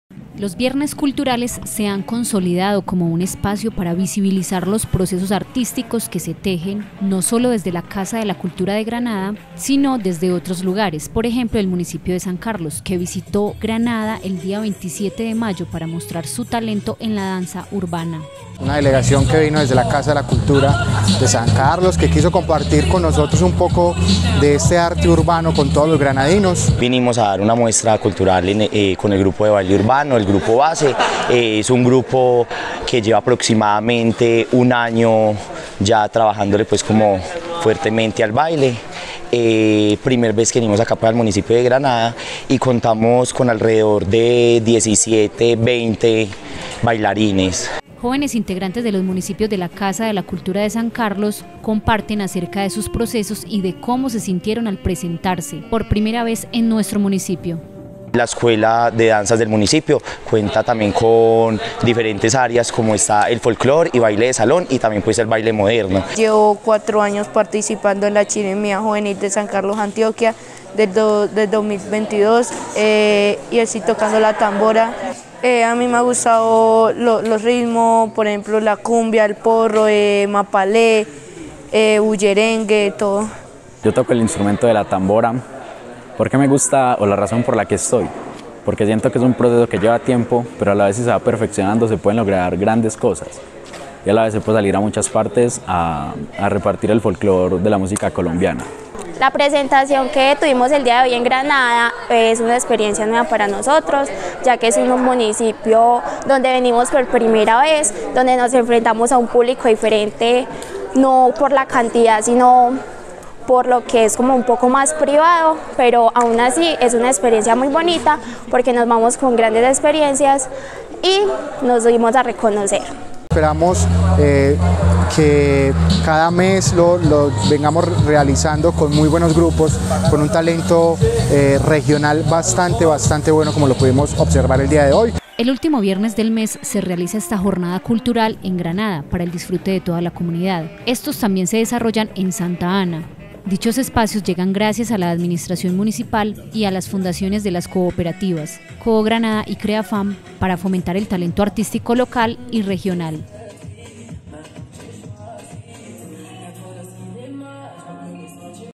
Delegación de la Casa de la Cultura de San Carlos participó del Viernes Cultural en Granada.
💃🎵| Desde la Casa de la Cultura del municipio de San Carlos, llegó una delegación de danza urbana para participar del Viernes Cultural; un espacio donde se propició el compartir entre ambos municipios y se pudo evidenciar las nuevas dinámicas de los jóvenes por medio de este género musical.

- Somos el Sistema de Televisión Comunitario de Granada - //ASOCOPAG//